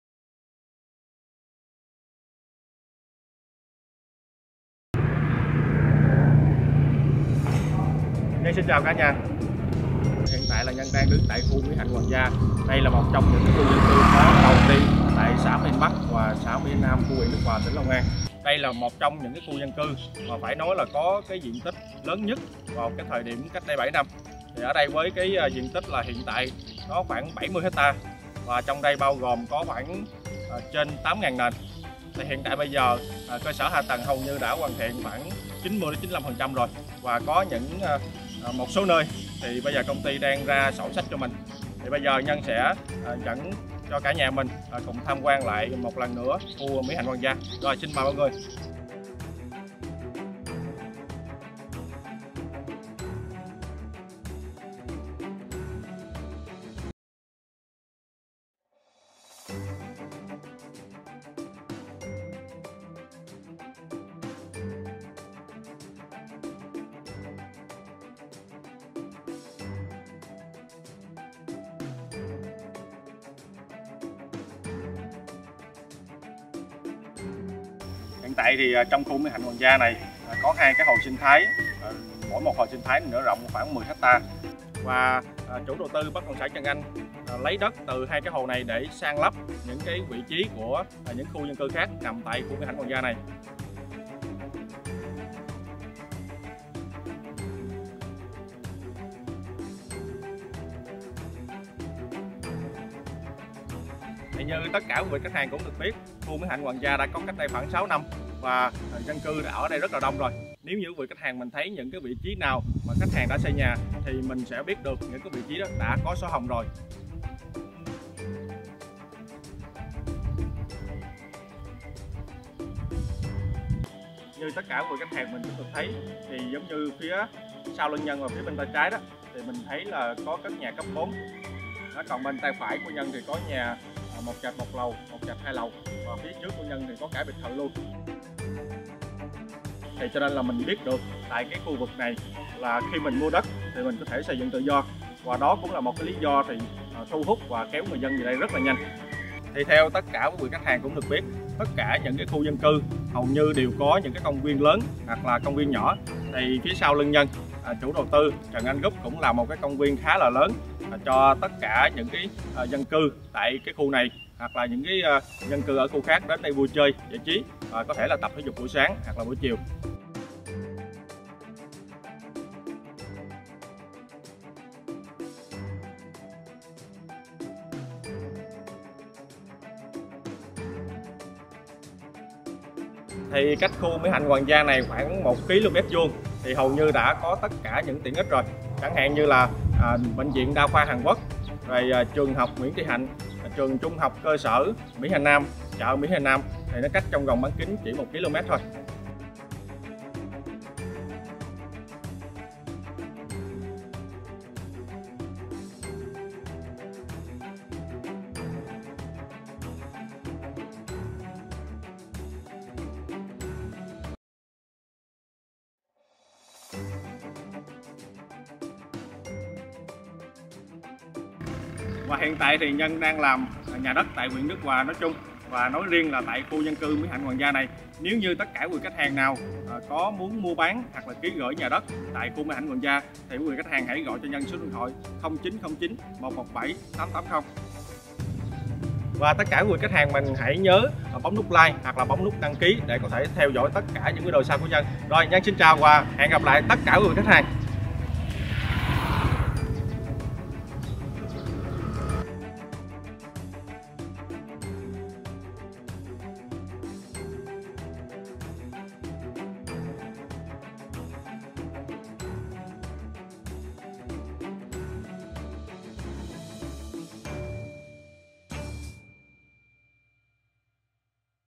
Nguyễn Trung Nhân 0909117880 l Khu Dân Cư Mỹ Hạnh Hoàng Gia Mỹ Hạnh Bắc, Đức Hòa Long An
- Nguyễn Trung Nhân sđt: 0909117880 ( zalo - viber - face book )

Đất nền ngay khu dân cư Mỹ Hạnh Hoàng Gia Tuyến đường huyết mạch của cửa ngõ Tây Bắc thành phố.

Đối diện có bệnh viện đa khoa Xuyên Á chi nhánh 2 đang xây dựng.

Liền kề nhiều KCN: Nhị Xuân, Xuyên Á, cụm CN Hoàng Gia, Hải Sơn.

Xung quanh khu dân cư đã có dân cư hiện hữu. Gần chợ, gần trường học cấp I, II, III.

Pháp Lý tốt: QĐ 1/500 SHR từng nền. Mở bán giai đoạn 1 giá tốt cho nhà đầu tư.

Tính thanh khoản cao, phù hợp với nhà đầu tư lướt sóng.

- Lợi nhuận của khách hàng, danh dự của chúng tôi.

Vị trí:

Nằm ngay trung tâm Mỹ Hạnh Nam, gần ngay ngã 3 mỹ hạnh, cách Sân Bay Tân Sơn Nhất 14km, nằm trong khu vực dân cư đông đúc.

Giá bán: 7tr/m2.

Diện tích: 90m2, 100m2, 120m2,...

Tiện ích: Nội khu: Bên trong dự án có trường học, cung thiếu nhi, khu vui chơi giải trí, công viên,...

Ngoại khu: Nằm trong khu vực trung tâm gần khu dân cư Mỹ Hạnh Hoàng Gia, Cát Tường Phú Sinh, chợ Mỹ Hạnh, bưu điện, phòng khám đa khoa, ủy ban nhân dân xã, KCN Đức Hòa Đông, xung quanh có nhiều nhà máy, xí nghiệp...